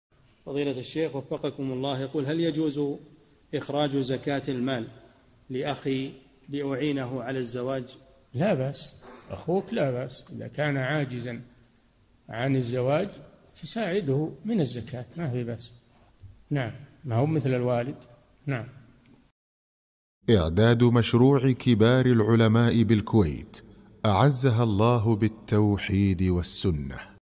[580 -582] هل يجوز للأخ أن يعطي زكاته لأخيه ليساعده على الزواج؟
-"السؤال: هل يجوز إخراج زكاة المال لأخي لأعينه على الزواج؟
الجَوَابُ: لا بأس أخوك لا بأس إذا كان عاجزًا عن الزواج تساعده من الزكاة ما في بأس ما هو مثل الوالد.
للاشتراك في قناة الشيخ صالح الفوزان
سؤال وجواب من شرح صفة الصلاة ( 582 ) مقطع

للاستفسار والإقتراحات والدعم
رئيس الفريق
لمشروع كبار العلماء